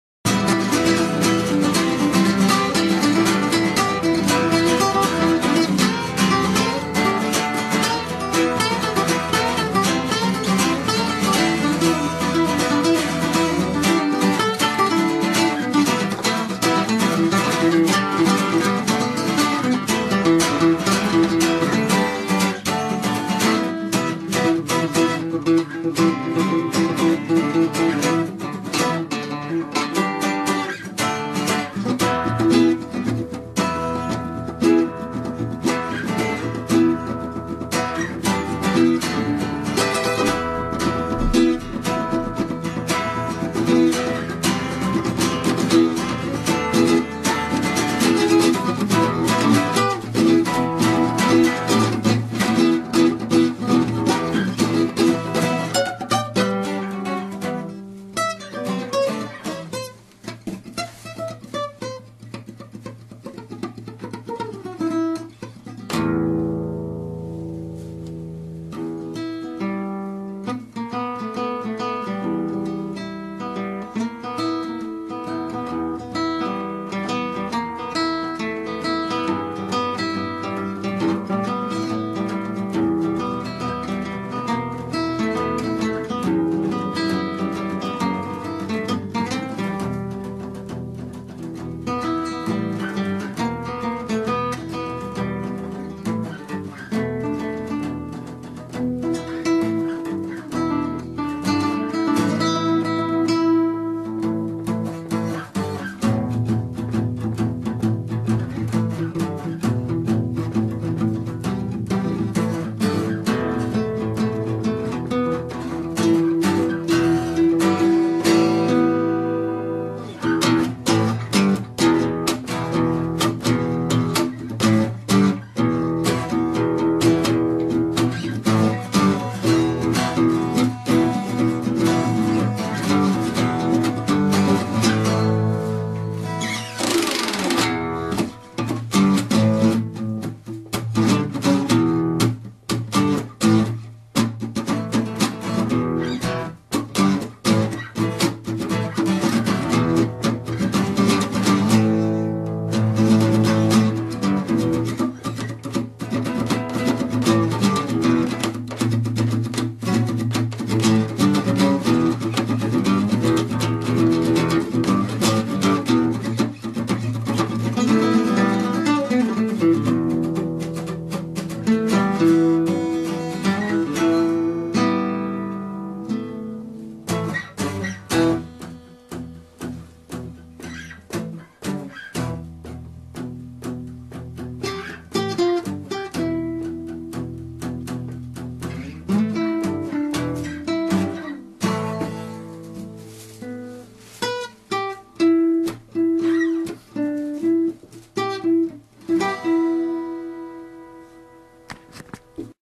Great Acoustic Jam - Sounds ... new oO'.
Hammer-geiler Jam von Timon und mir und unseren beiden Akustikgitarren. Ich hab' total mit Akkorden und Anschlägen experimentiert. In einer Art, dass ich es wieder von dieser Aufnahme lernen müsste. ...
Am Anfang spiel ich "Solo" und später Begleitung (interessant)